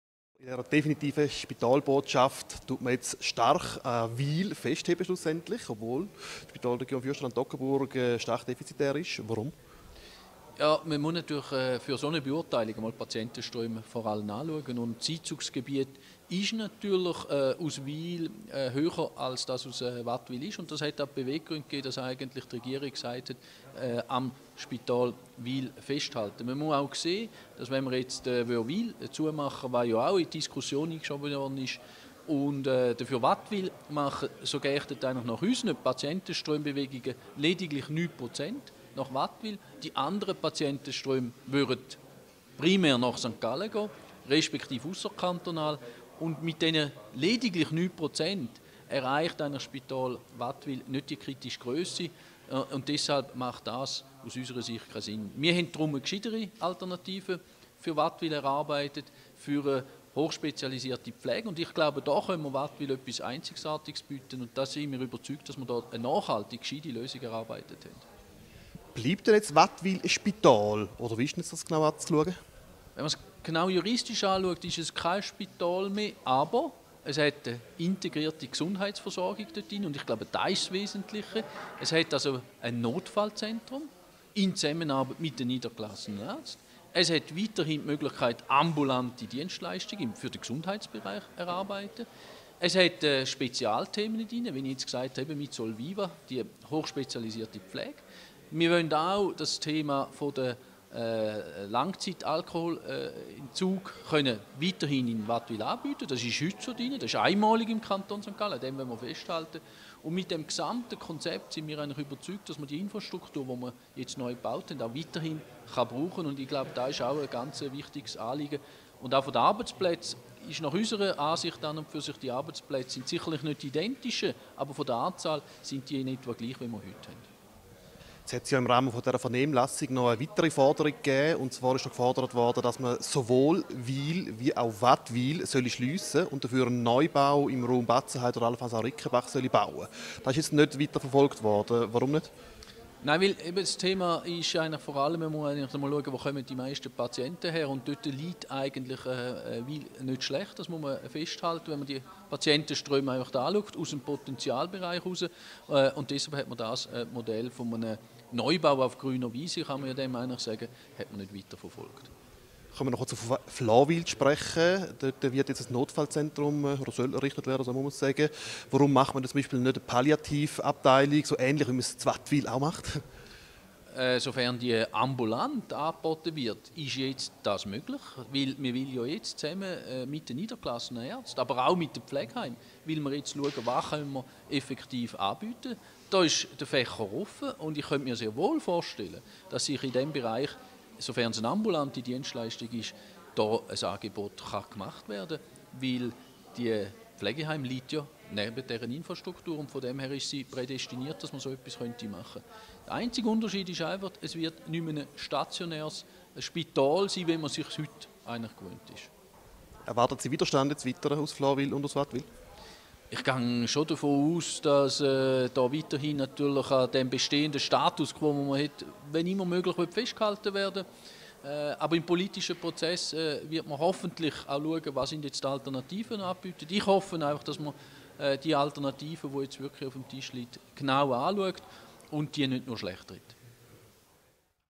Marc Mächler im Interview
Der Regierungsrat erklärt, warum es keine Option war, Wattwil offen zu lassen und Wil zu schliessen.